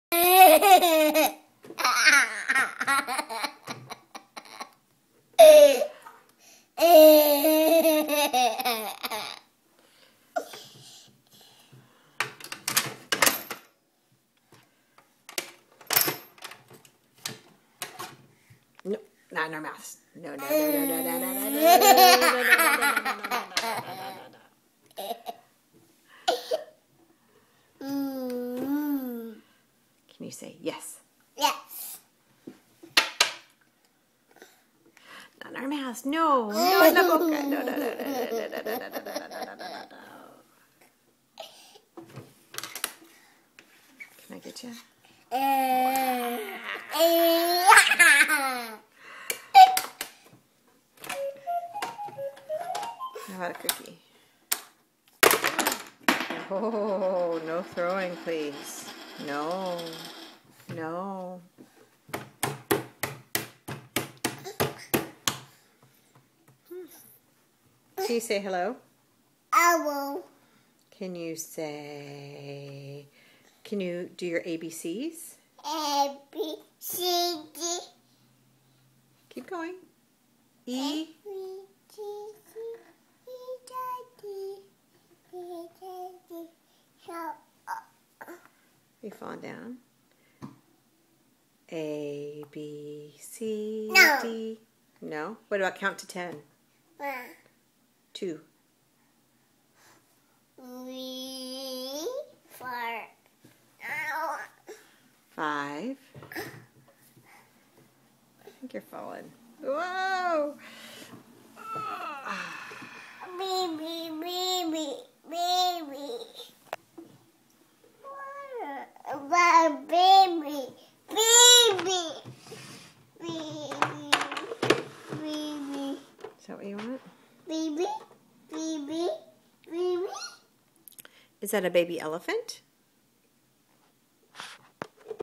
William laughing
Giggles, counting, and some play